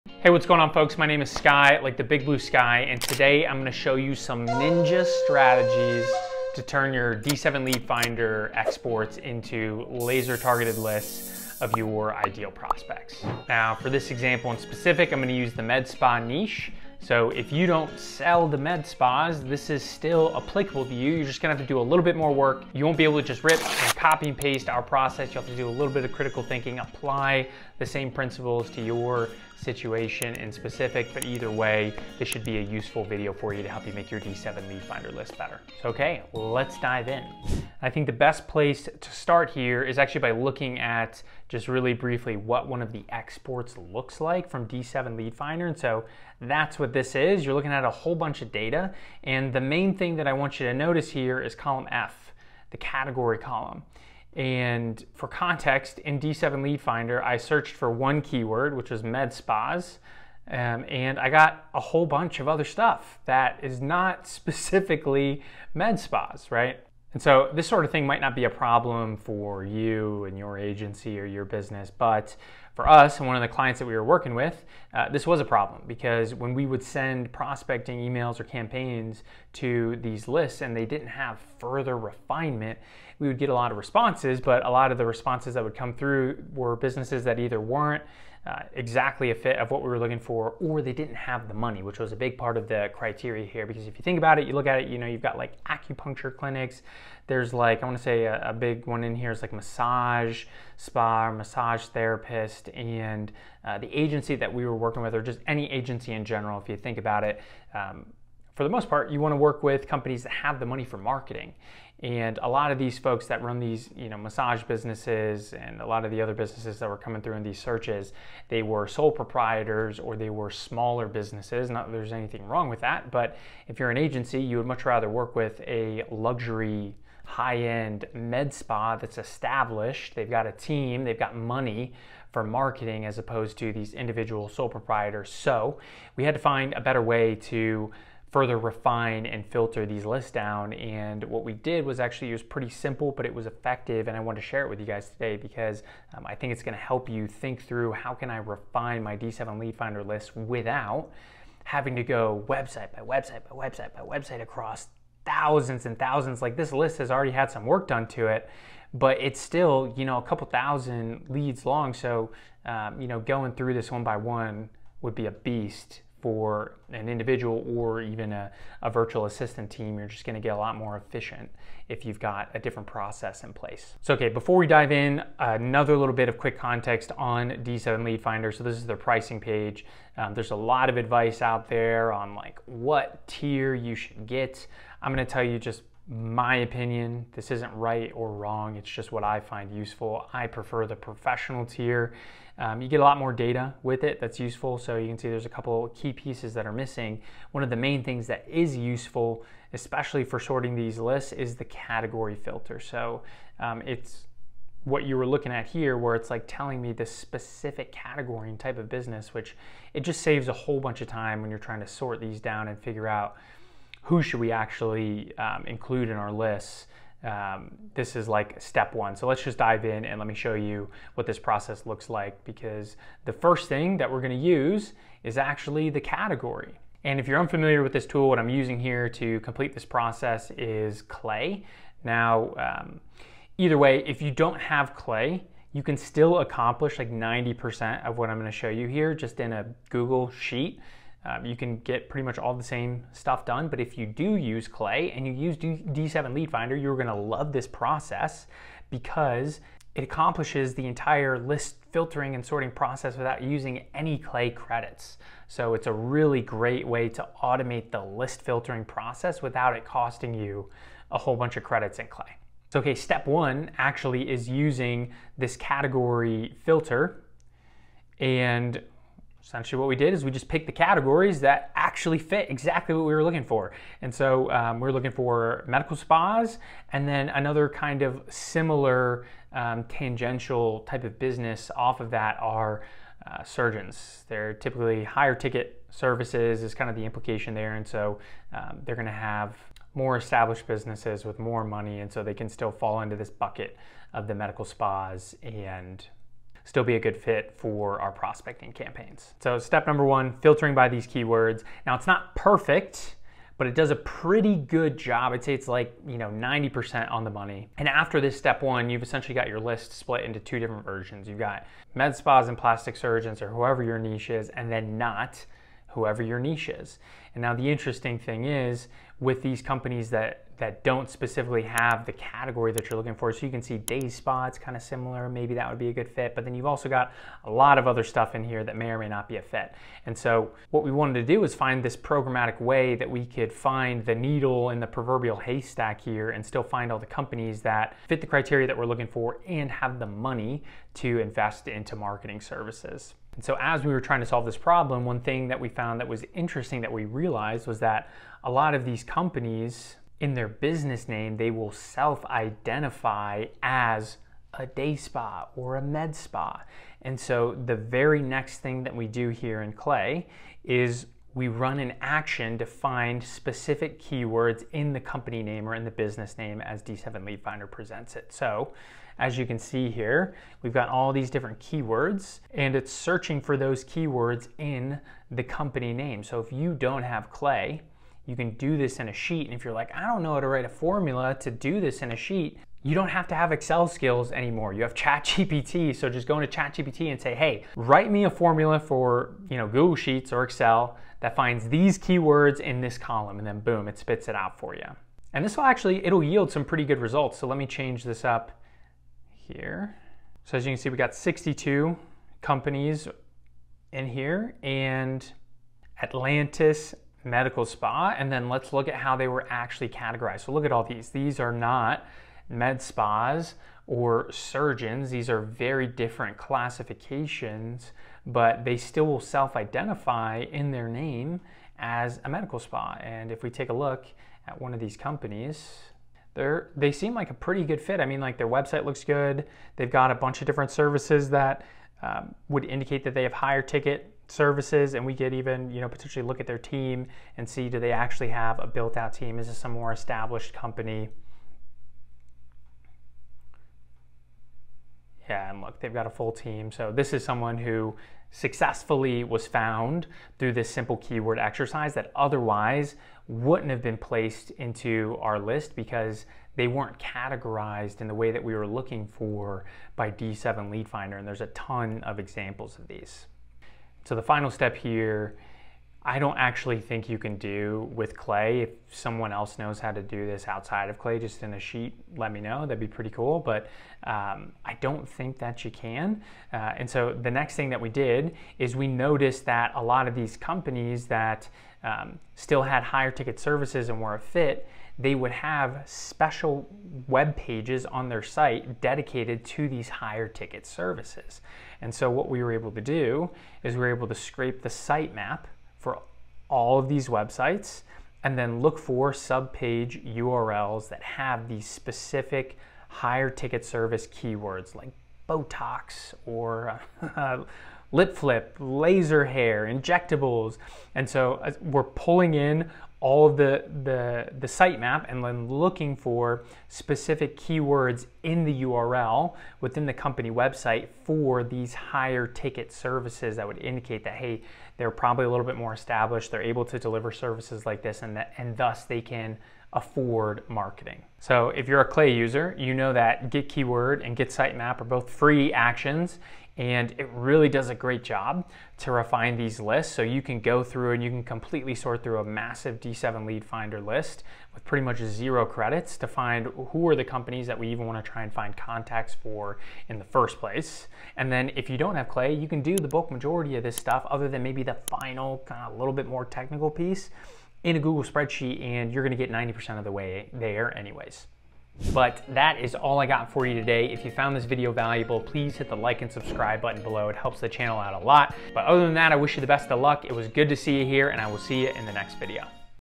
How To Build Laser Targeted Lists With D7 Lead Finder | Complete Tutorial
✅ EXERCISE FOR YOU

How To Build Laser Targeted Lists With D7 Lead Finder

In today's video, I'm excited to share with you an incredible tool called D7 Lead Finder that will revolutionize how you build laser-targeted lists.

If you're tired of wasting time and effort on generic leads, this is the solution you've been waiting for.

I'll walk you through step-by-step on how to effectively utilize D7 Lead Finder to discover highly relevant and qualified leads for your business.

By harnessing the power of this tool, you'll be able to create customized outreach campaigns that yield higher conversions and greater success.

So, let's dive in and unlock the potential of building laser-targeted lists with D7 Lead Finder!